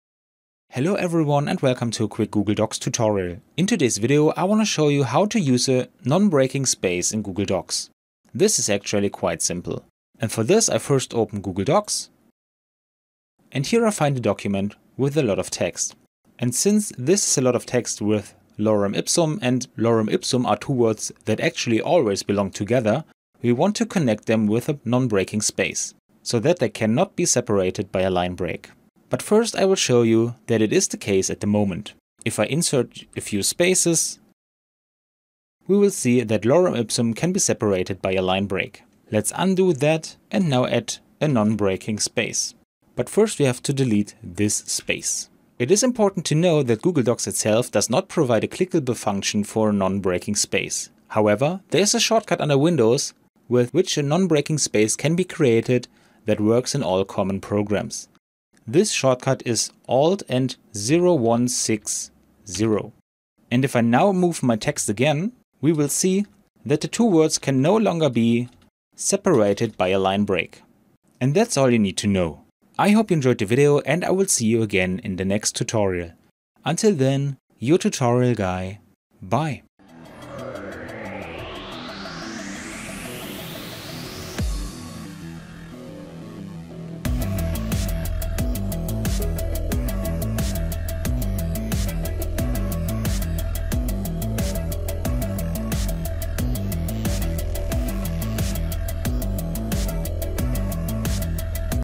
Non breaking space in Google Docs ✅ Tutorial
Learn how to easily add non-breaking spaces in Google Docs with this step-by-step tutorial. Non-breaking spaces are useful when you want to prevent words or characters from breaking at the end of a line. Watch this video to master this simple yet handy feature in Google Docs!